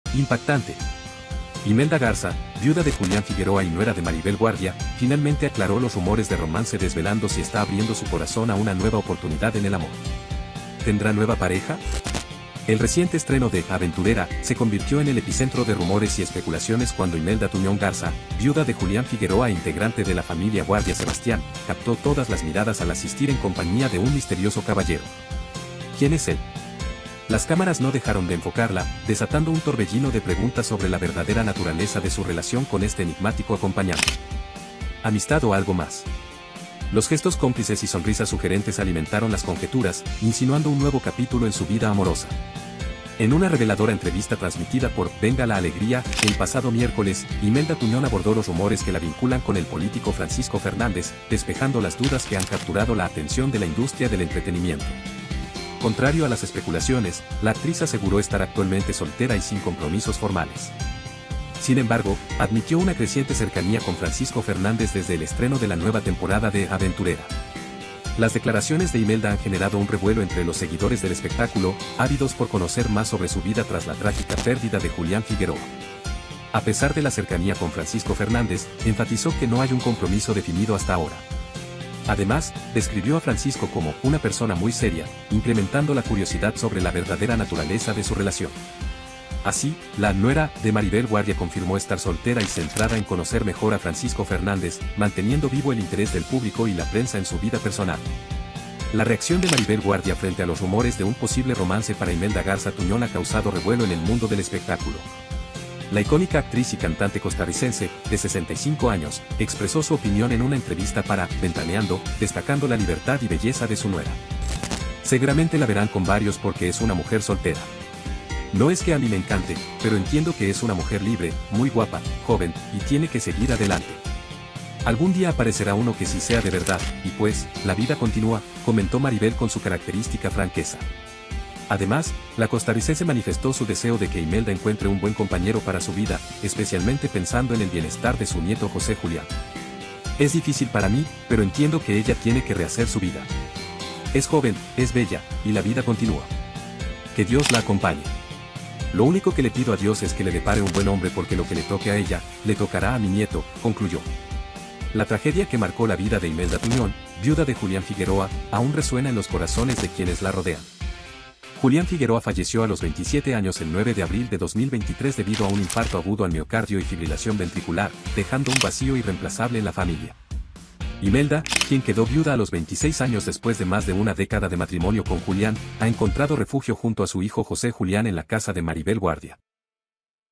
Viuda de Julián Figueroa aclara rumores de romance: ¿Se está dando una nueva oportunidad en el amor?
Viuda de Julián Figueroa aclara rumores de romance y revela si se está dando una nueva oportunidad en el amor